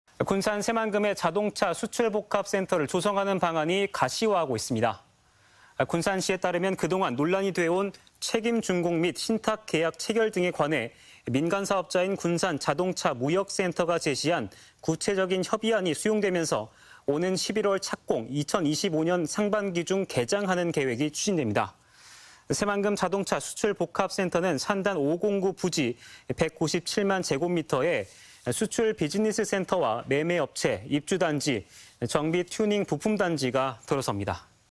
새만금자동차수출복합센터 11월 착공 추진 | 전주MBC 230801 방송
임홍진 기자의 더 많은 기사를 보고싶다면?

군산 새만금에 자동차 수출복합센터를 조성하는 방안이 가시화하고 있습니다.

군산시에 따르면 그동안 논란이 돼온 책임준공 및 신탁계약 체결 등에 관해 민간사업자인 군산자동차무역센터가 제시한 구체적인 협의안이 수용되면서 오는 11월 착공, 2025년 상반기 중 개장하는 계획이 추진됩니다.

새만금자동차수출복합센터는 산단 5공구 부지 197만 제곱미터에 수출비즈니스센터와 매매업체 ,입주단지, 정비.튜닝.부품단지가 들어섭니다.